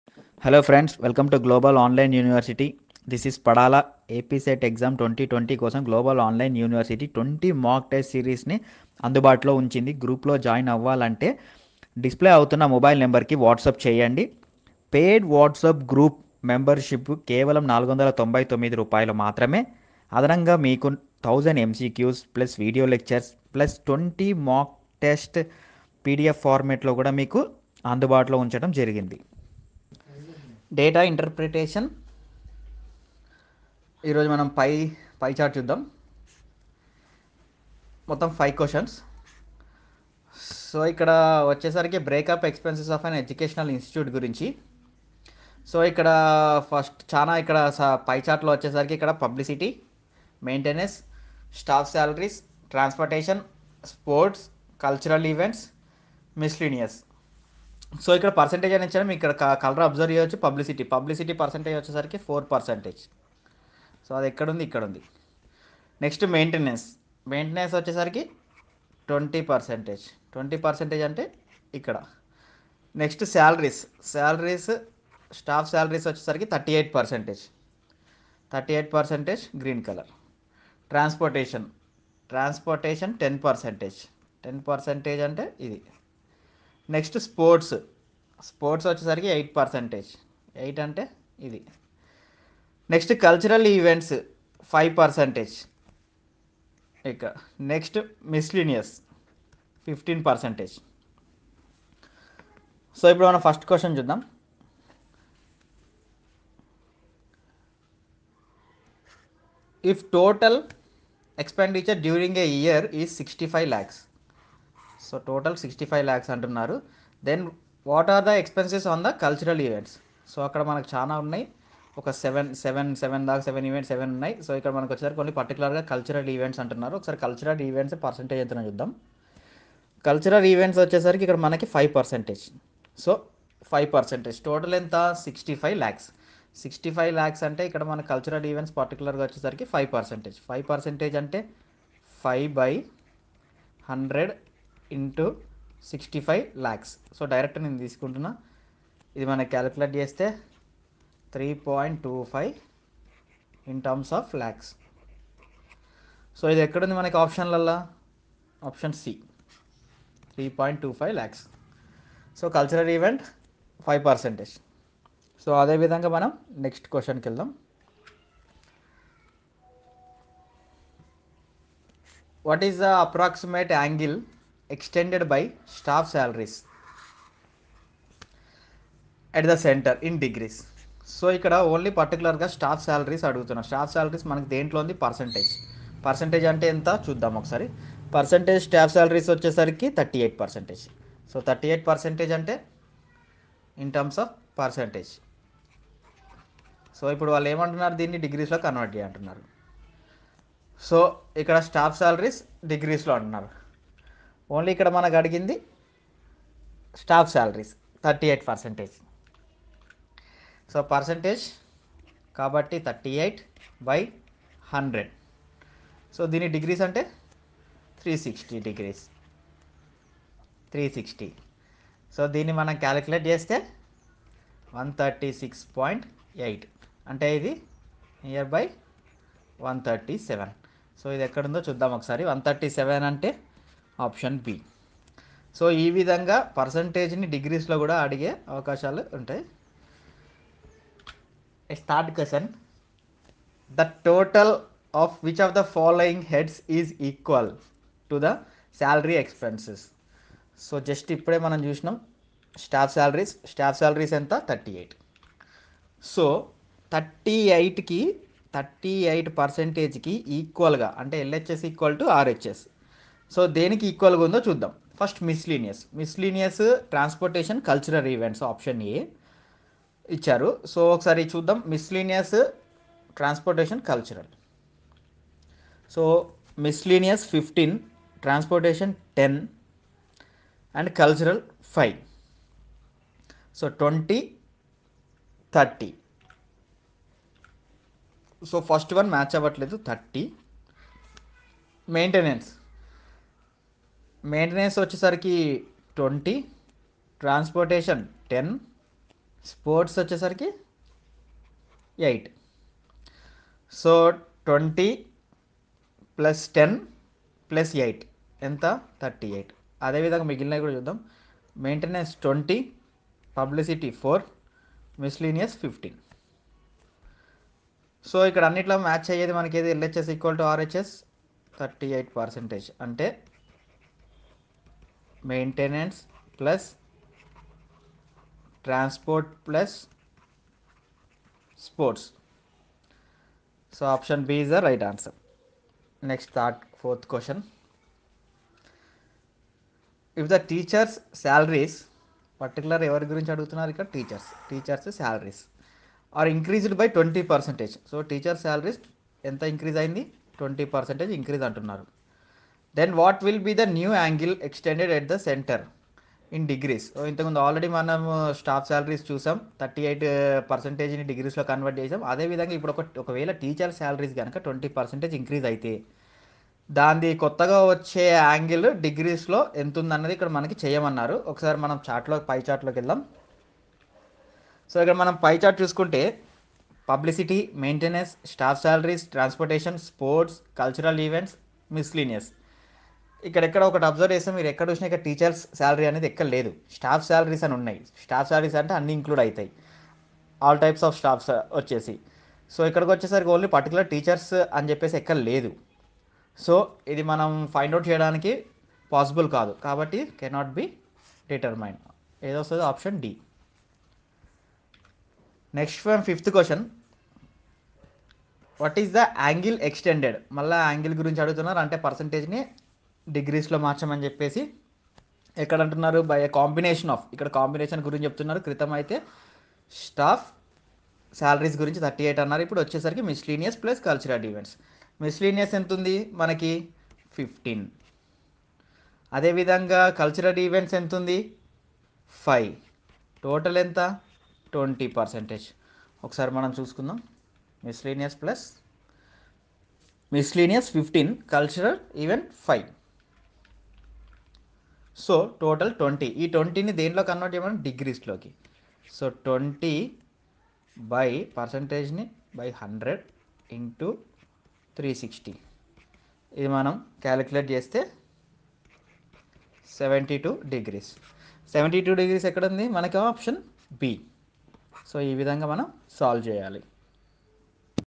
APSET Paper 1 Data Interpretation 2020 | Most Important Questions in Telugu Mcqs NTA UGC NET
In This Video we have discussed about Data Interpretation Which Are Very Useful for Andhra Pradesh State Eligibility Test Examination 2020(AP SET EXAM 2020). Questions Are Solved With Very Clear Explanation In Telugu And Also With Tips And Tricks To Boost Your Preparation. UGC NTA NET 2020
వద్ద డౌన్‌లోడ్ చేసుకోవచ్చు.
డౌన్‌లోడ్ చేసుకోవడానికి మా యాప్ క్లిక్ చేయండి హెట్ప్స్: // చెల్లింపు కోర్సు కోసం మా వెబ్‌సైట్‌ను సందర్శించండి
Andhra Pradesh state eligibility test apset – 2020 general paper on teaching & research aptitude syllabus paper-1